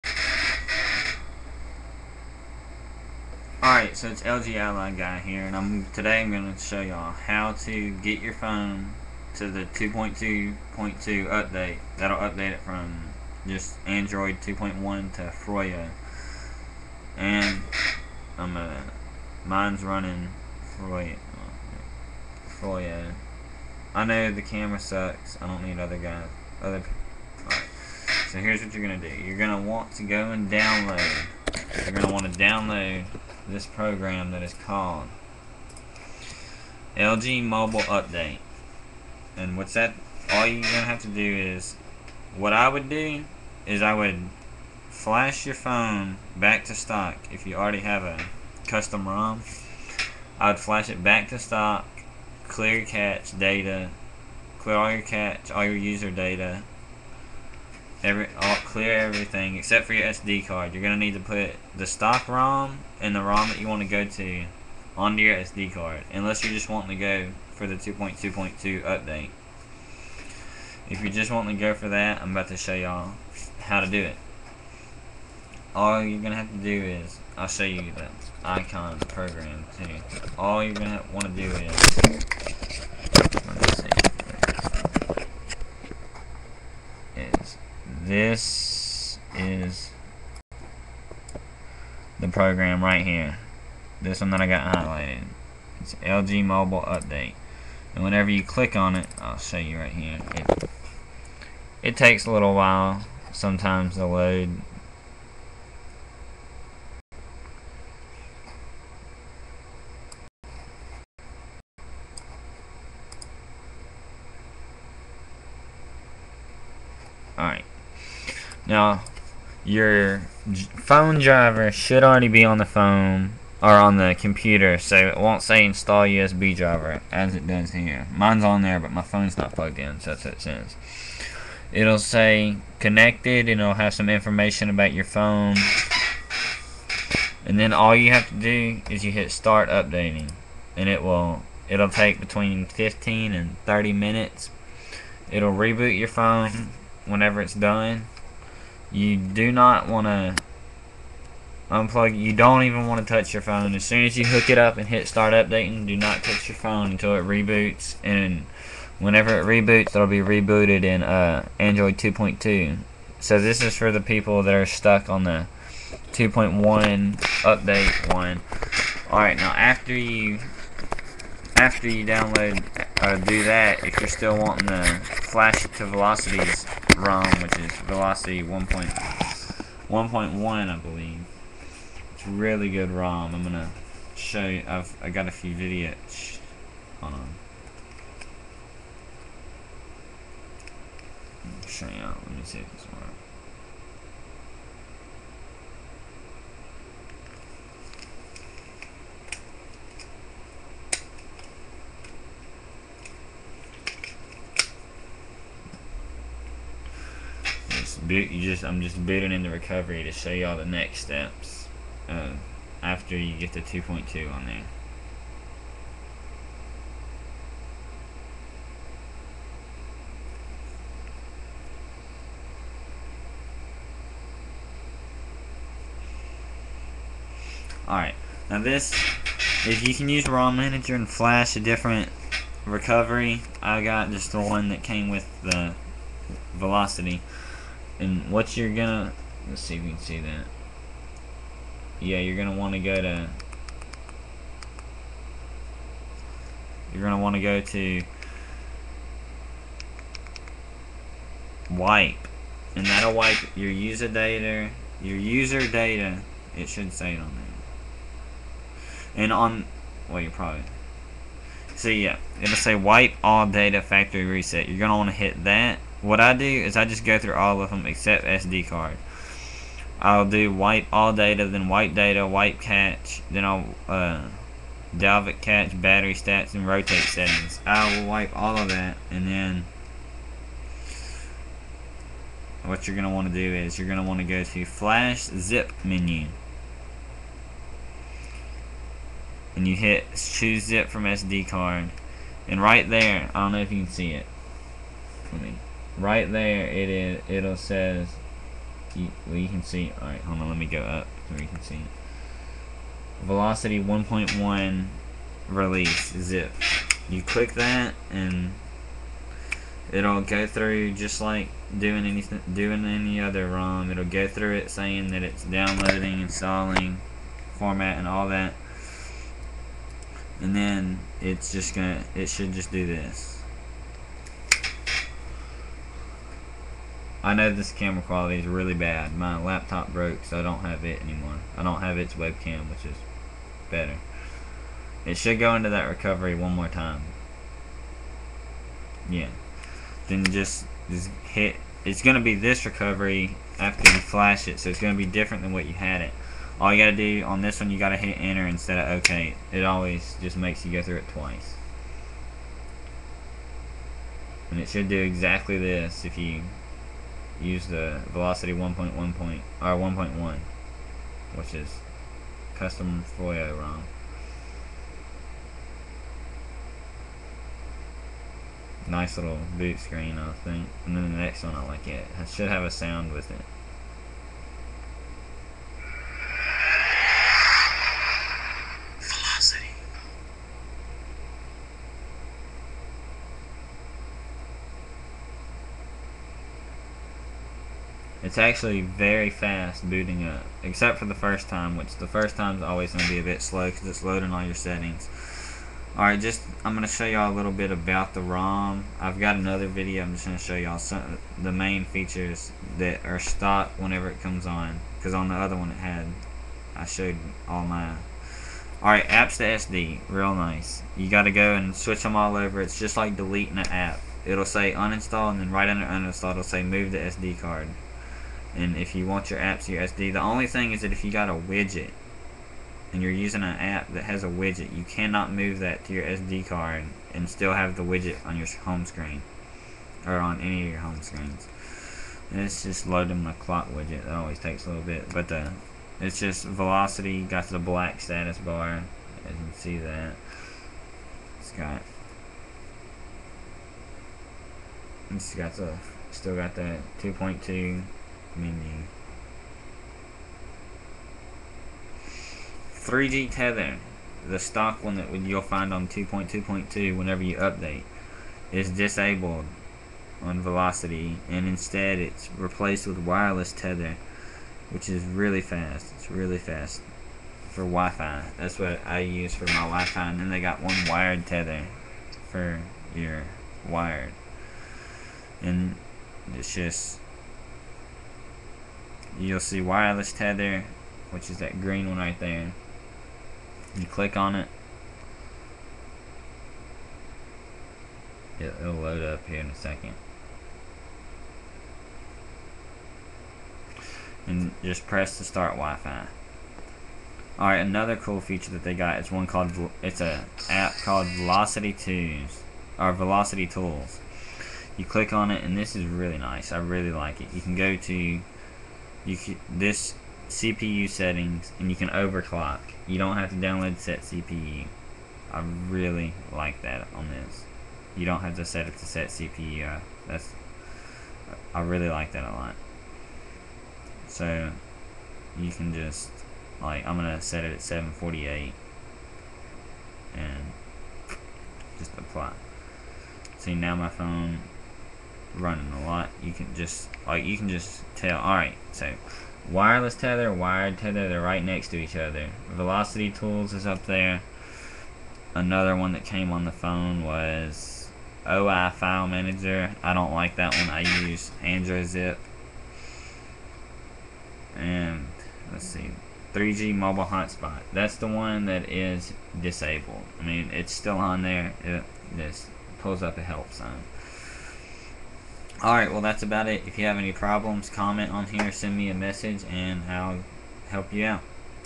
How To Force Update Lg Ally To Android 2,2,2 And Flash Velocity 1.1
This Is A Video Of How To Force update You Lg Ally From Android 2.1update1 to Android 2.2.2 Froyo
Heres The Link To Lg Mobile Update

click the update button and do not touch the phone until it says 100% Complete